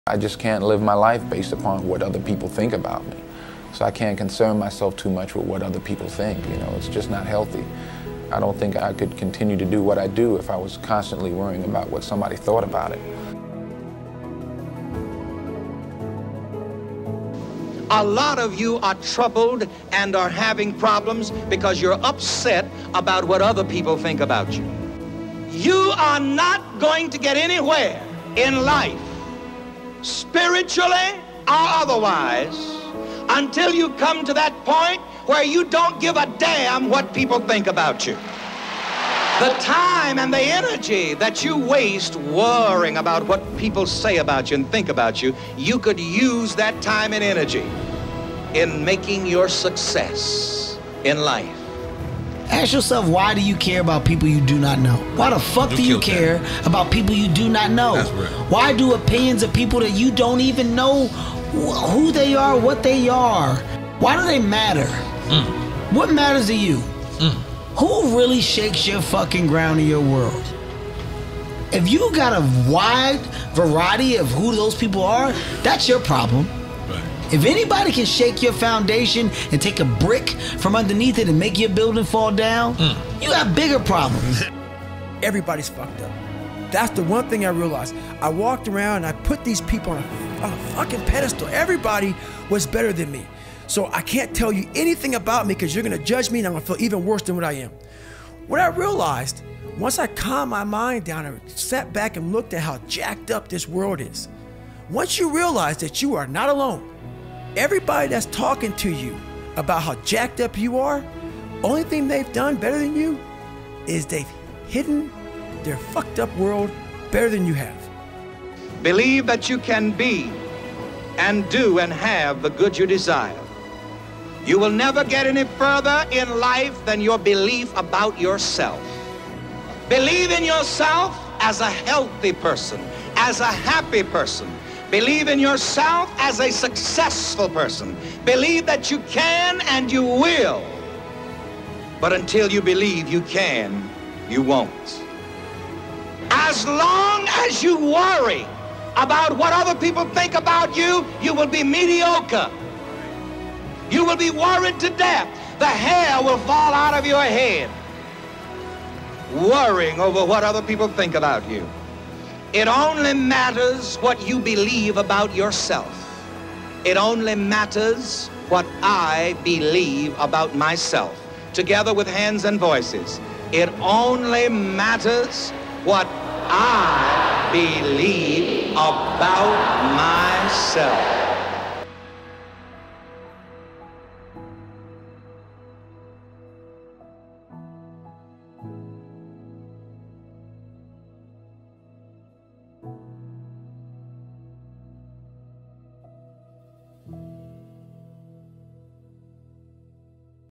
STOP WORRYING WHAT OTHER PEOPLE THINK ABOUT YOU |This Is Why Most People Fail In Life
In this inspiring video, four successful and influential figures - Denzel Washington, Kevin Hart, David Goggins, and Rev. Ike - share their insights on the importance of letting go of other people's opinions and embracing your authentic self. Learn how to overcome fear, negative self-talk, and judgement, and live your life fearlessly.

Motivational Video | Motivational Speech | Inspirational Video | Life Success